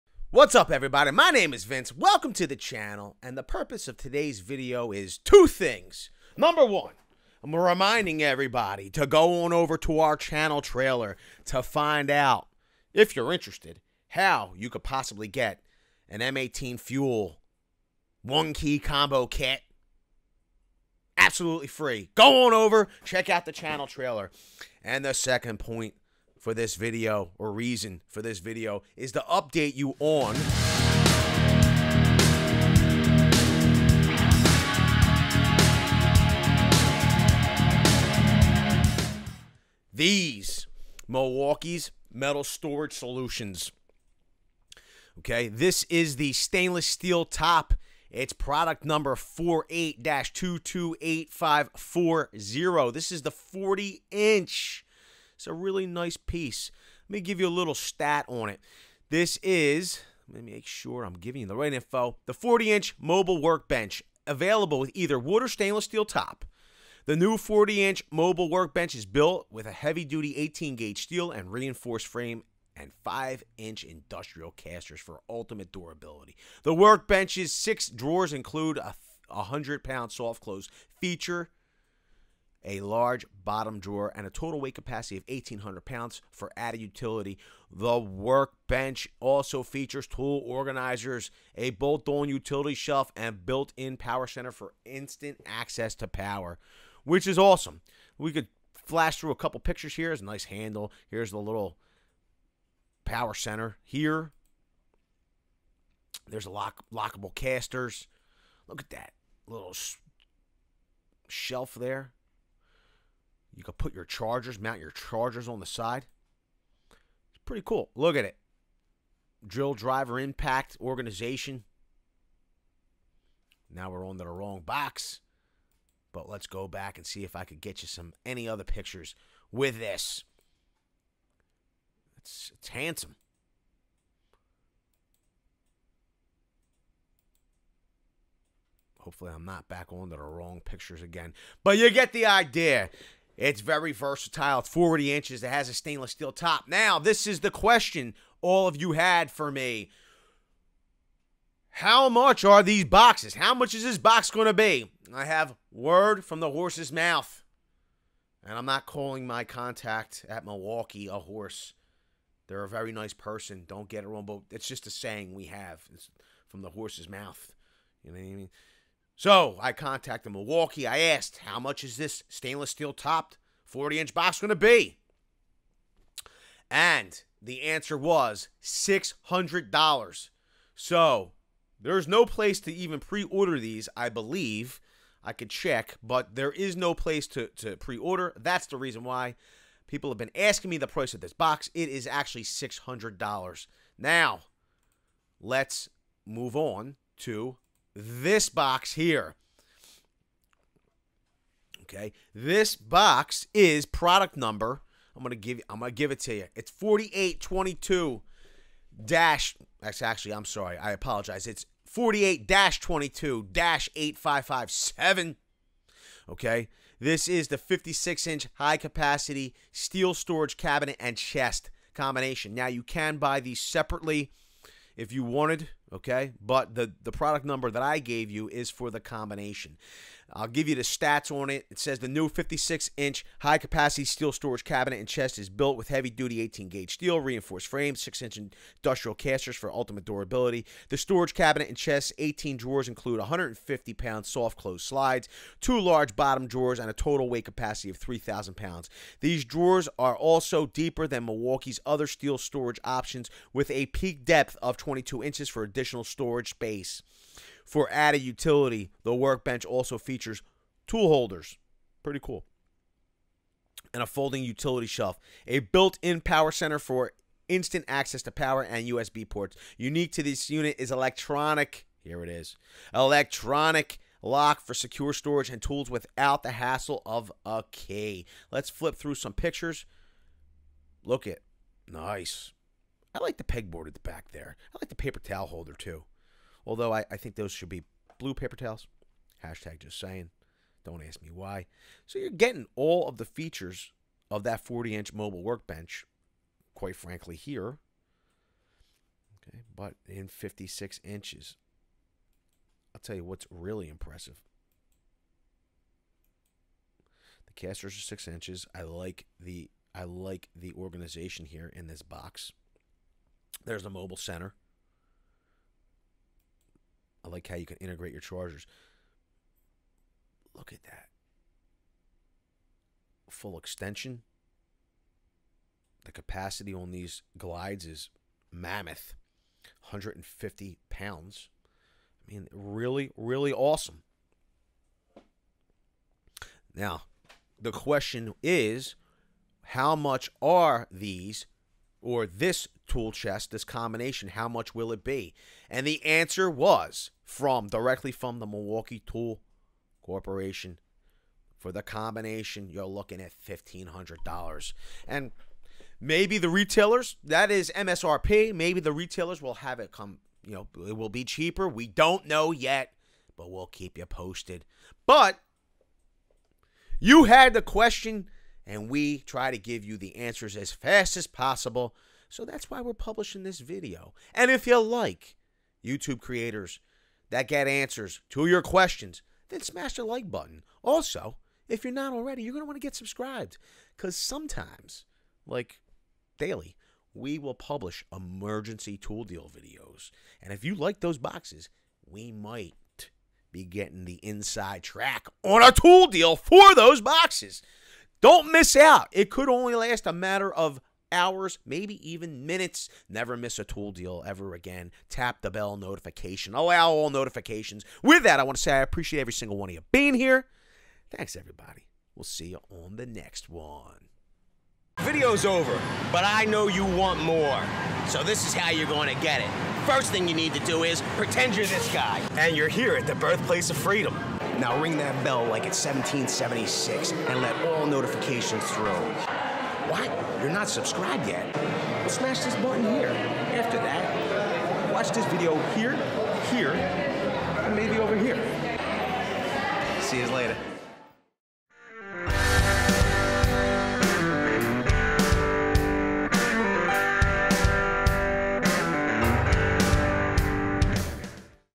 Newest Milwaukee Steel Storage Solution YOU WONT BELIEVE THE PRICE
Newest Milwaukee Steel Storage Solution YOU WONT BELIEVE THE PRICE of the Milwaukee 56” High Capacity Steel Storage Cabinet & Chest (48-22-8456) and 40” Mobile Work Bench (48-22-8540) We give the VeryCoolGang the discount tooldeal price !